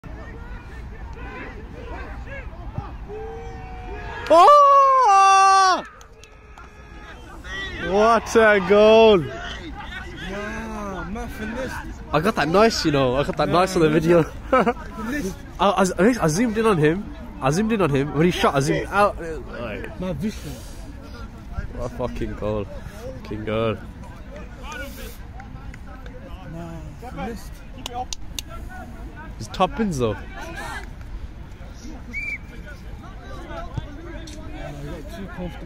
Vish Scores Worldie Against Village FC
Vishav Kumar scores a worldie playing for Nechells Athletic in the Birmingham and District League

Full Match HIghlights Available on the Channel